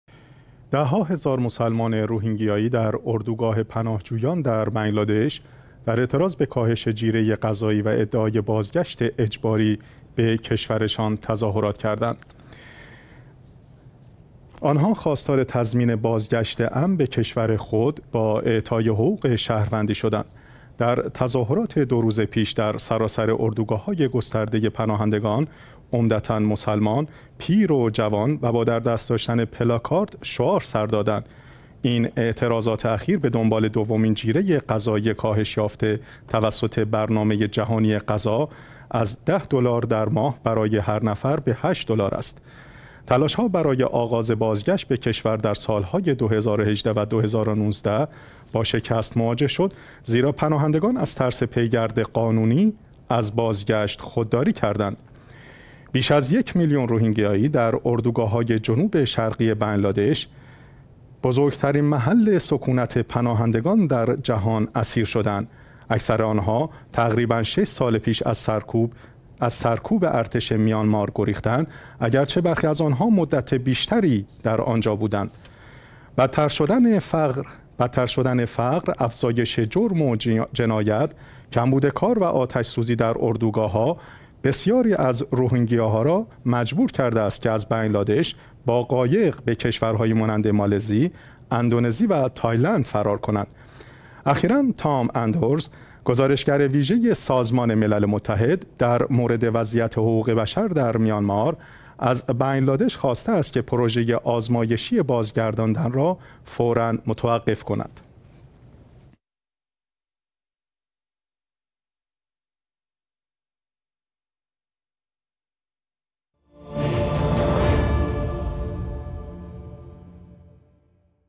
تظاهرات مسلمانان روهینگیایی برای بازگشت امن به میانمار
ده‌ها هزار مسلمان روهینگیایی در اردوگاه پناهجویان در بنگلادش، در اعتراض به کاهش جیره غذایی و ادعای بازگشت اجباری به کشورشان تظاهرات کردند. آنها خواستار تضمین بازگشت امن به کشور خود با اعطای حقوق شهروندی شدند.
در تظاهرات دو روز پیش، در سراسر اردوگاه‌های گسترده، پناهندگان عمدتا مسلمان، پیر و جوان، پلاکاردها را تکان دادند و شعار سر دادند. این اعتراضات اخیر به دنبال دومین جیره غذایی کاهش یافته توسط برنامه جهانی غذا از 10 دلار در ماه برای هر نفر به 8 دلار است.
تلاش‌ها برای آغاز بازگشت به کشور در سال‌های 2018 و 2019 با شکست مواجه شد زیرا پناهندگان از ترس پیگرد قانونی، از بازگشت خودداری کردند.
بیش از یک میلیون روهینگیایی در اردوگاه‌های جنوب شرقی بنگلادش، بزرگترین محل سکونت پناهندگان در جهان، اسیر شده ‌اند. اکثر آنها تقریباً شش سال پیش از سرکوب ارتش میانمار گریختند، اگرچه برخی از آنها مدت بیشتری در آنجا بودند.
بدتر شدن فقر، افزایش جرم و جنایت، کمبود کار و آتش سوزی در اردوگاه ها، بسیاری از روهینگیاها را مجبور کرده است که از بنگلادش با قایق به کشورهایی مانند مالزی، اندونزی و تایلند فرار کنند.
اخیراً تام اندروز، گزارشگر ویژه سازمان ملل متحد در مورد وضعیت حقوق بشر در میانمار، از بنگلادش خواسته است که پروژه آزمایشی بازگرداندن را فوراً متوقف کند.